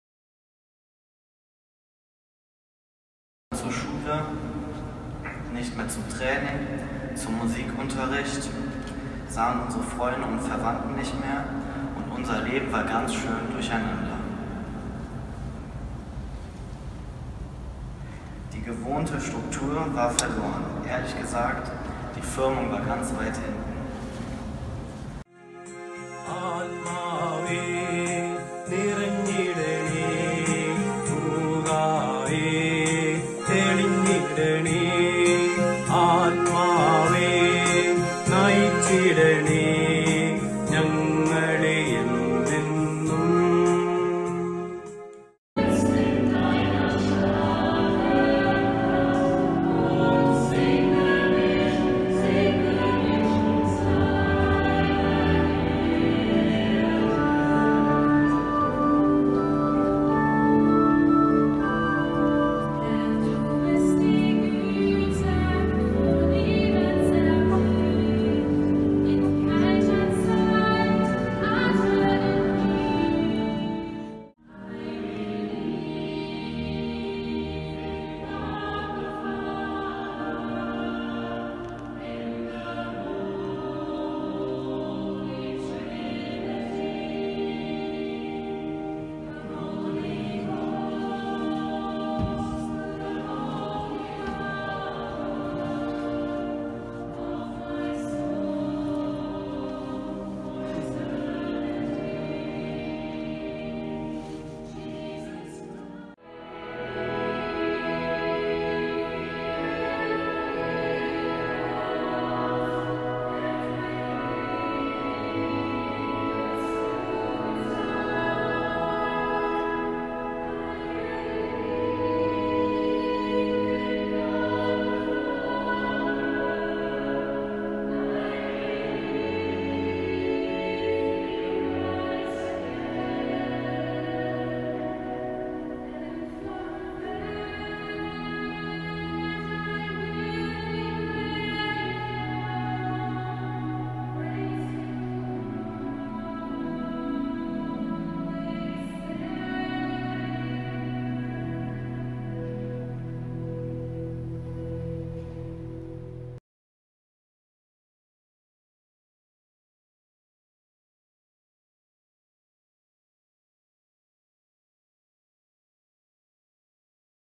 Firmung im Kölner Dom am 6.7.2020 - ein kleiner Einblick
Ein Teil unserer Firmkandidaten aus St. Servatius Siegburg nahm das Angebot wahr und ließ sich im Kölner Dom firmen.